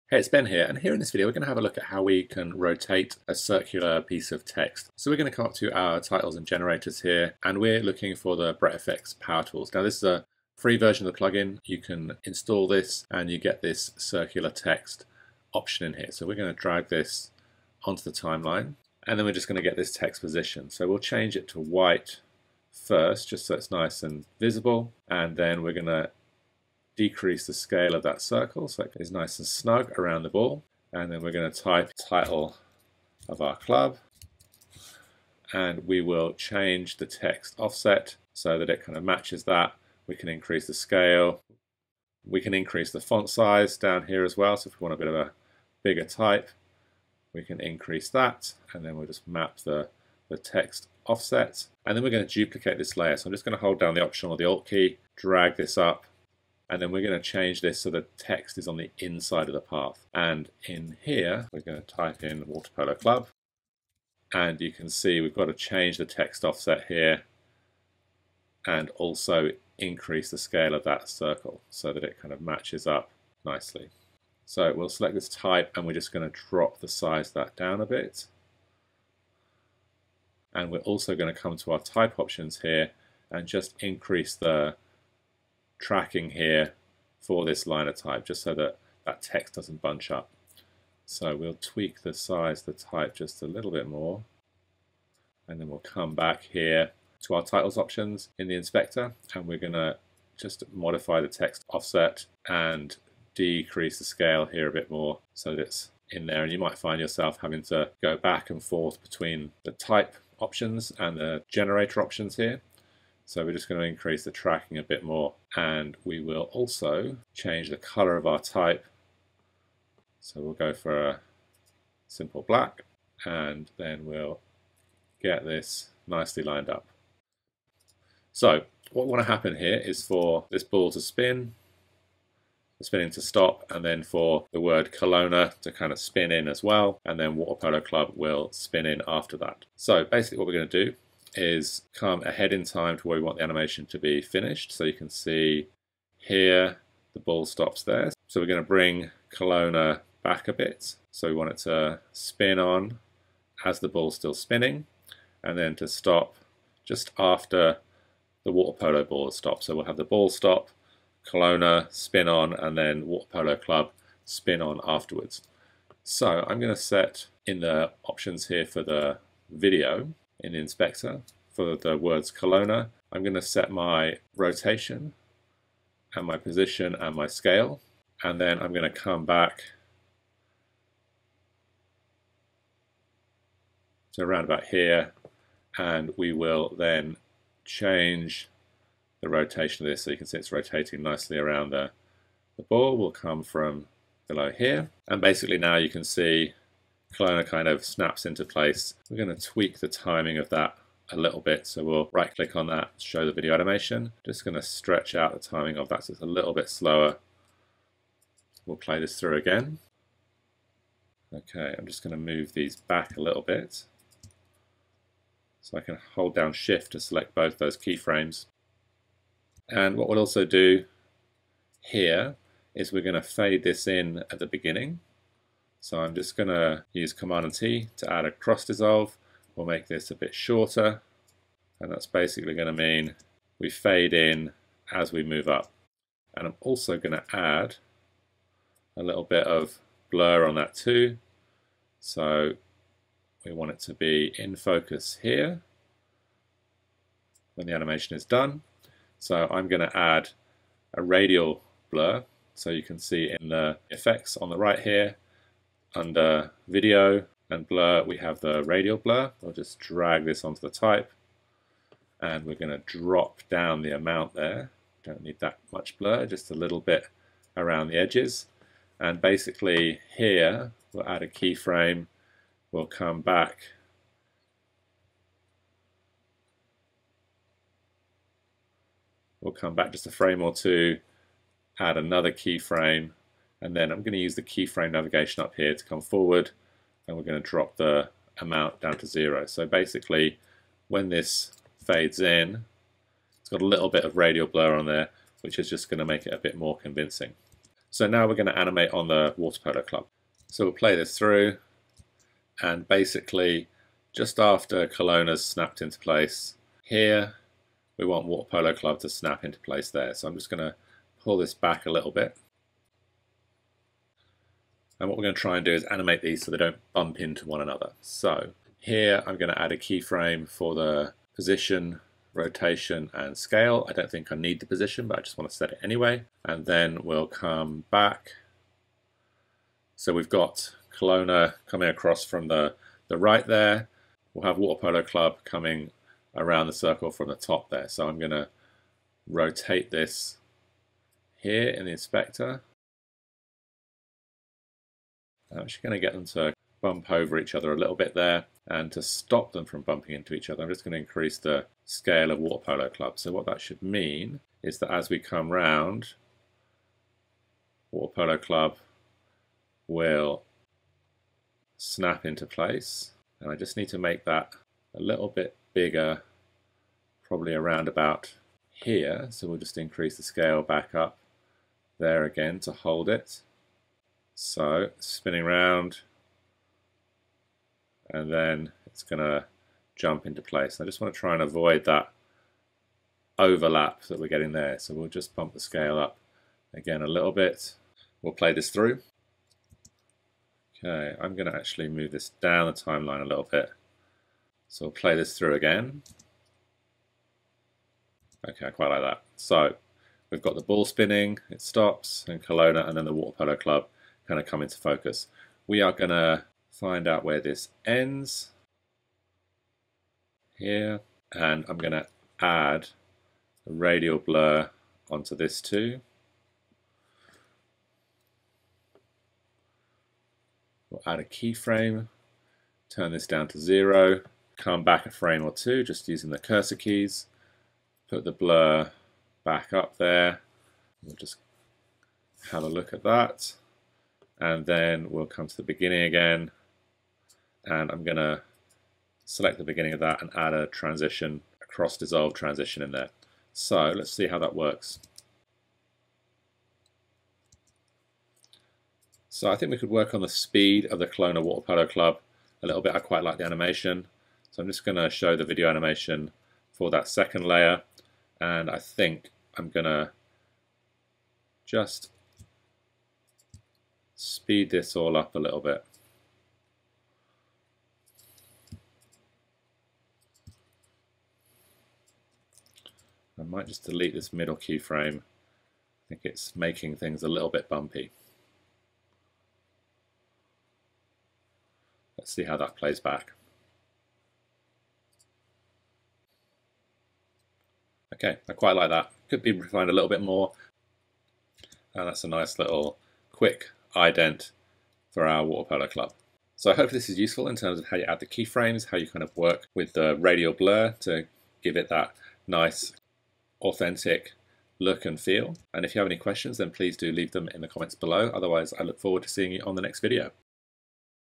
Final Cut Pro Tutorial | Wrap and Animate Circular Type
Wrap circular type and animate it in Final Cut Pro using keyframes.